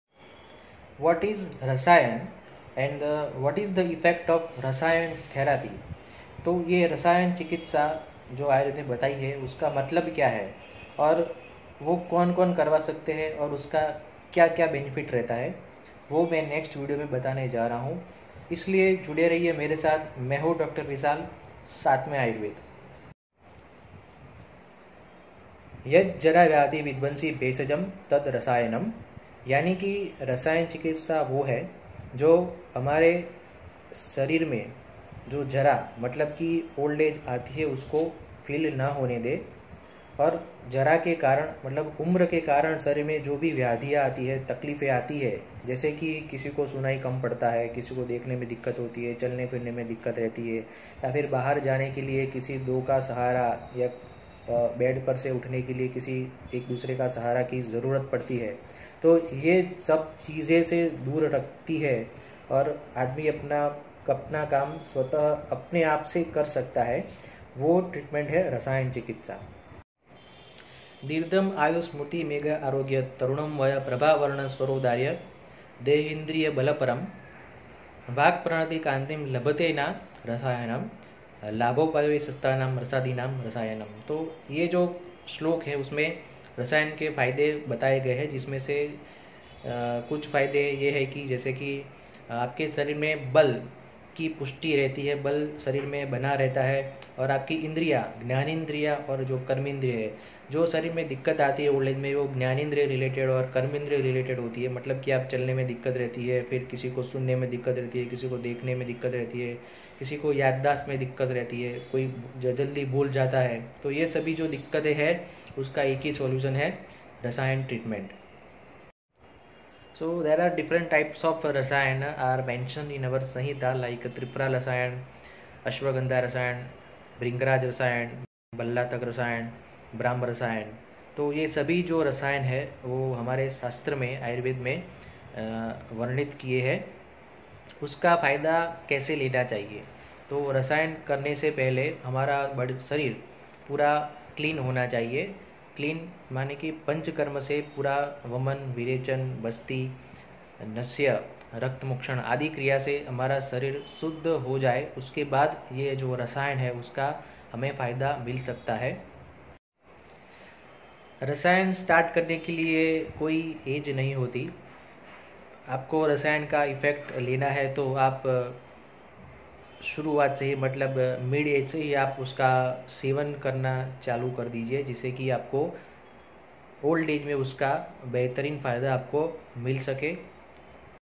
What is Rasayan?Effects of Rasayan.Benefits Of Rasayan | SECRETS TO HEALTHY AGING|HOW TO BE YOUNGER
Benifits of Rasayan treatment for healthy aging.who do not wants to stay young at old age, every one wants it, so I give small discription in video  regarding rasayan treatment mentioned in our ancient Ayurved Text.
 In old age our indriya -Gyanendriya,Karmendriyas related functions deteriorate,not work efficiently Thats why problem occurs.
Through Ayurveda we can manage this type of inability via Rasayan  as early as possible in our  life.
So it is not for older ones but also for younger once. Stay healthy in old age# Ayurved and Health # STAY YOUNGER#digestive
r